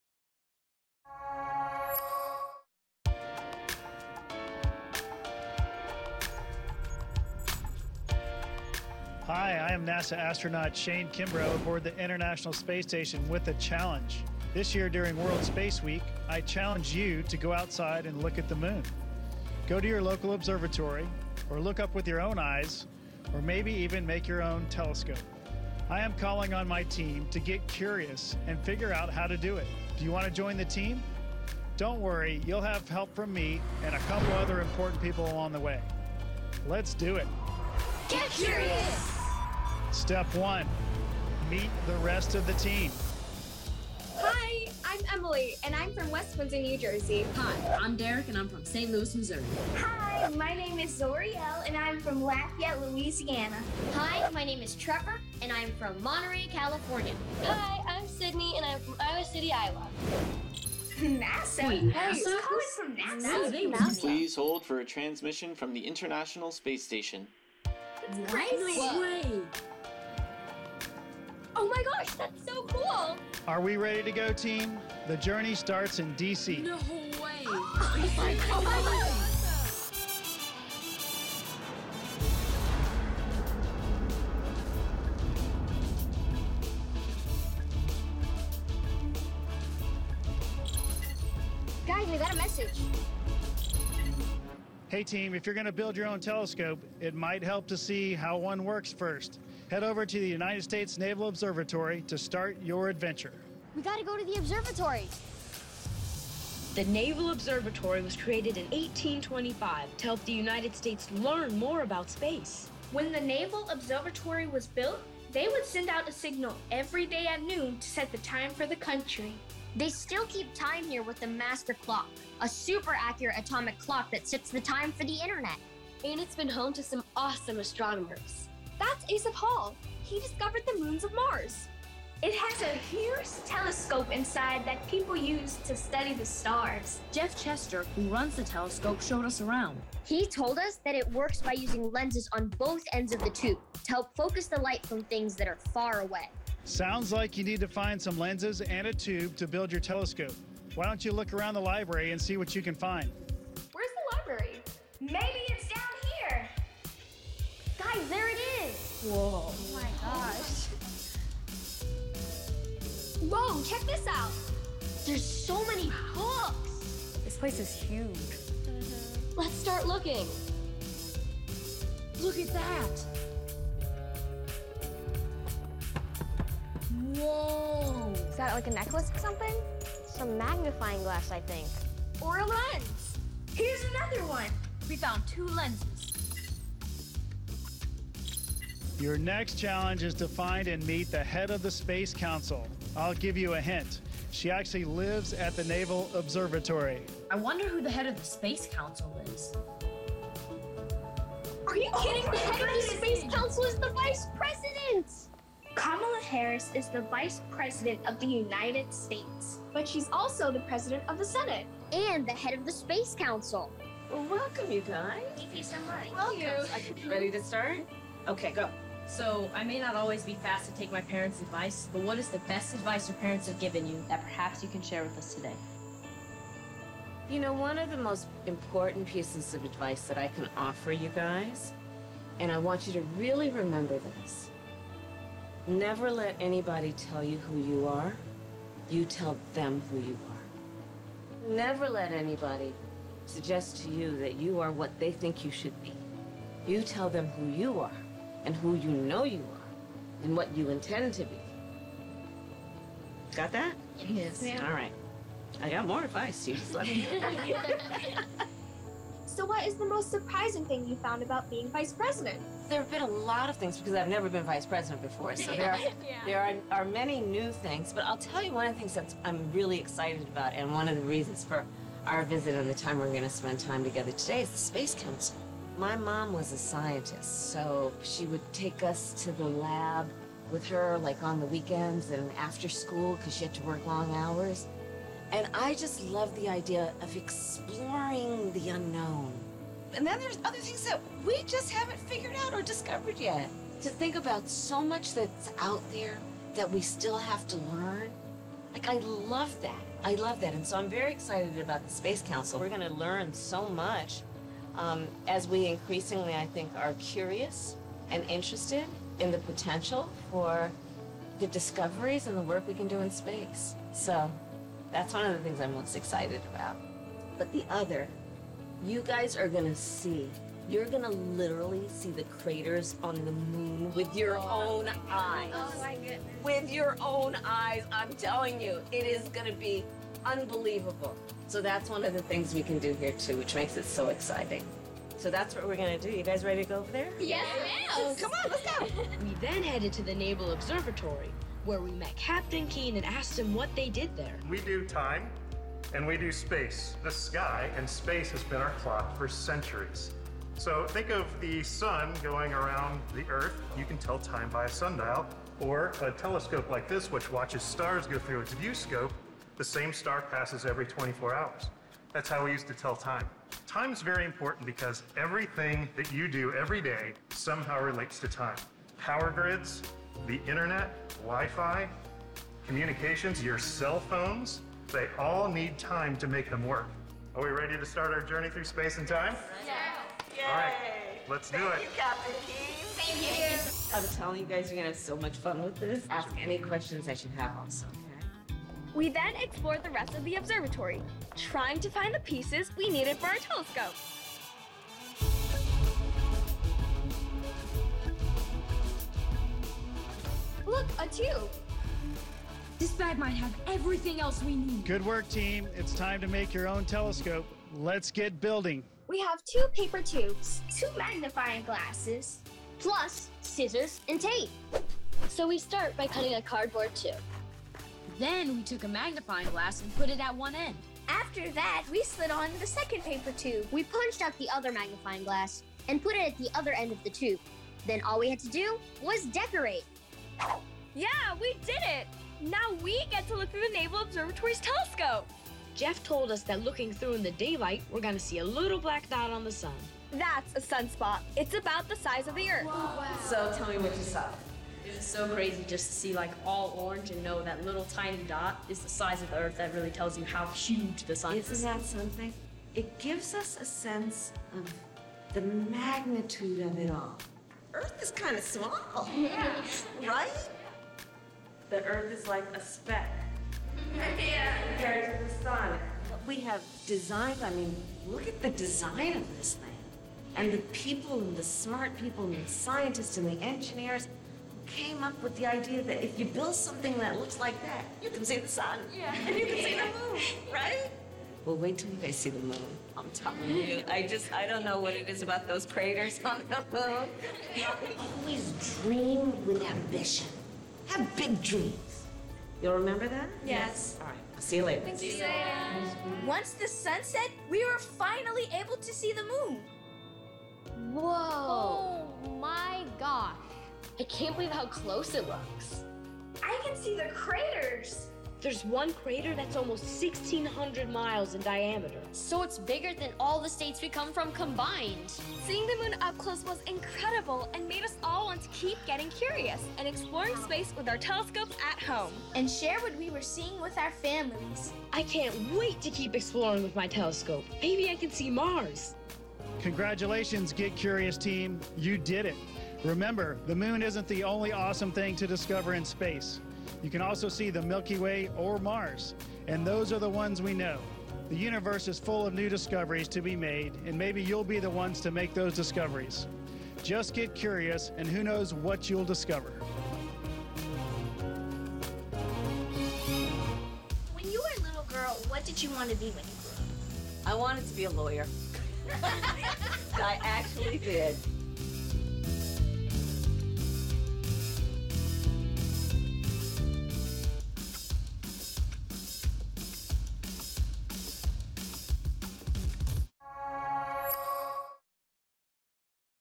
Vice President Kamala Harris and an Astronaut? What A Day! | Get Curious with Vice President Harris
In honor of World Space Week, we’re visiting the Naval Observatory in Washington to talk to none other than Vice President Kamala Harris about the National Space Council. We also hear from NASA astronaut Shane Kimbrough, who joins us from the International Space Station - that’s right, from space! Plus, we’re building a DIY telescope that you can make yourself at home.

To learn more about space-related educational activities, head over to nasa.gov/stem.